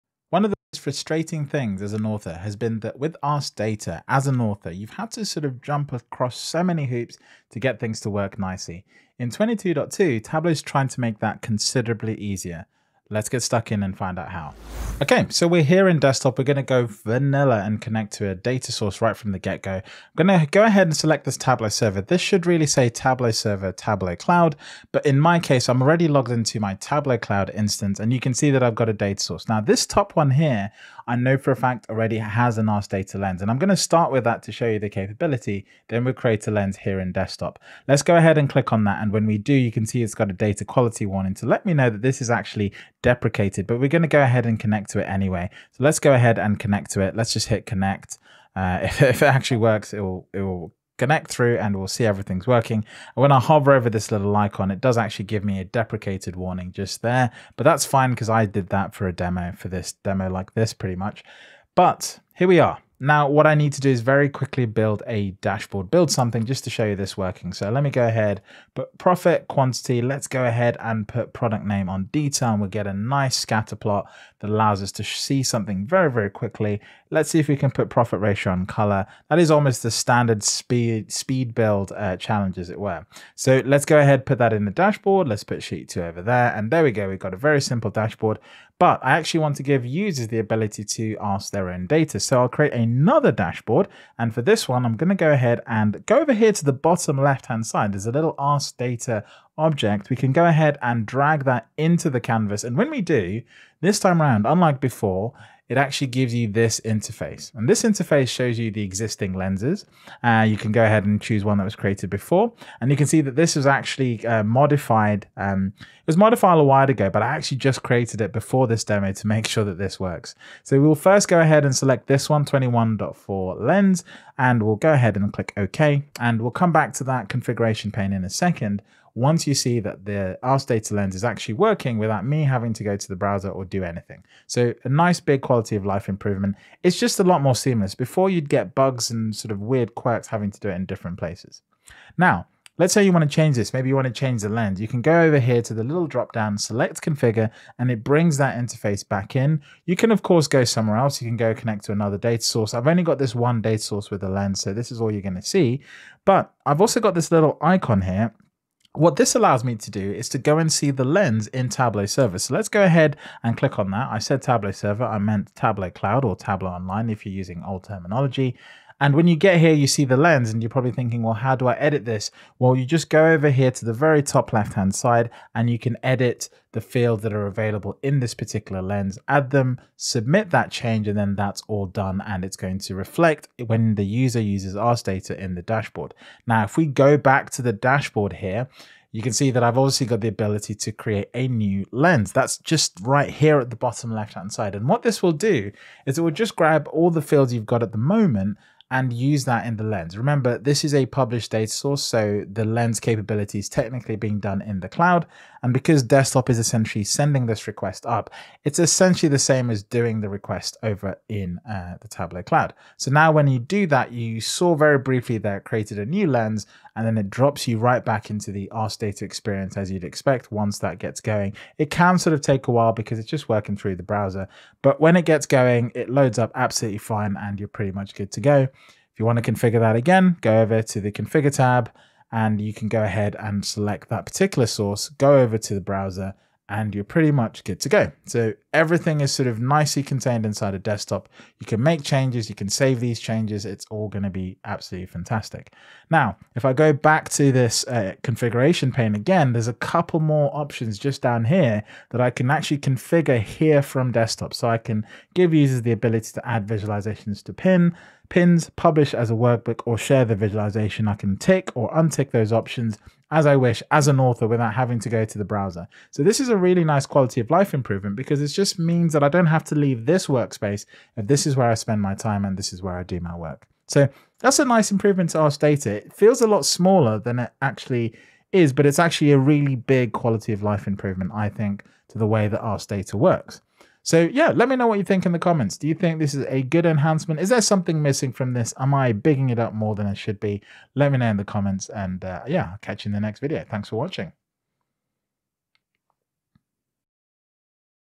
Ask Data Improvements - New in Tableau 2022.2
My Courses on Linkedin Learning We continue to extend the availability of Ask Data! Adding Ask Data to a Dashboard is now smoother than ever, supported end-to-end in Tableau Desktop. We are also increasing flexibility to manage Ask Data inside dashboards. Analysts can decide to show or hide the Ask Data Configuration toolbar, so business users can easily add visualisations to Pins, publish them as workbooks, or even share them with others.